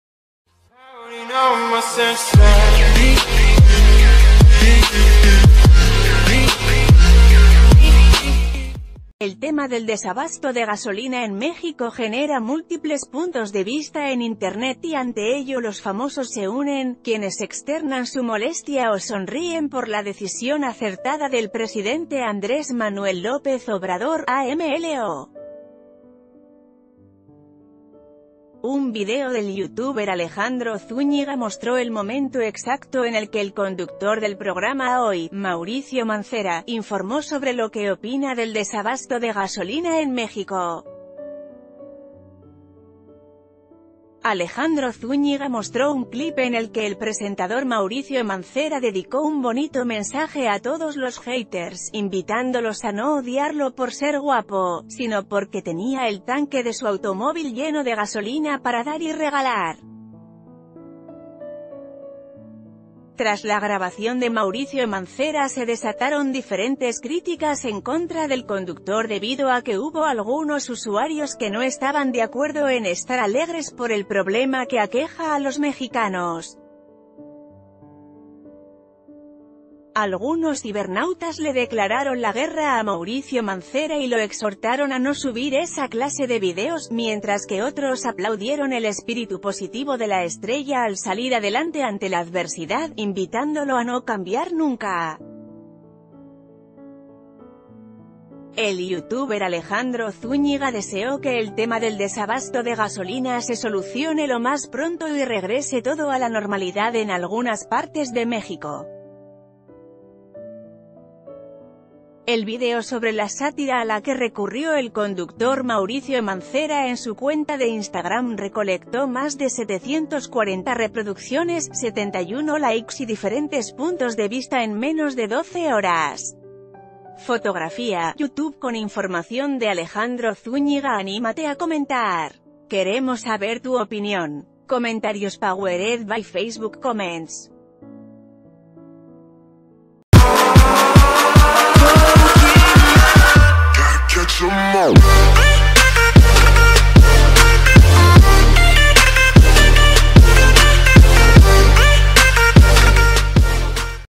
Mauricio Mancera desata críticas por burlarse del desabasto de combustible
El tema del desabasto de gasolina en México genera múltiples puntos de vista en internet y ante ello los famosos se unen, quienes externan su molestia o sonríen por la decisión acertada del presidente Andrés Manuel López Obrador (AMLO). Un video del YouTuber Alejandro Zúñiga mostró el momento exacto...